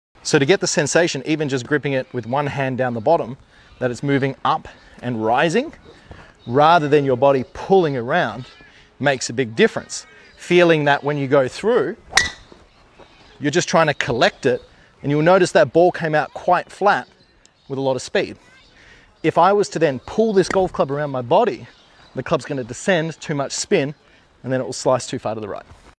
The Perfect Driver Delivery
If you’re struggling off the tee; hitting pop-up shots and slices this is a great feel to build in the sensation of getting the club head rising through the moment of impact and letting the club head release past the hands.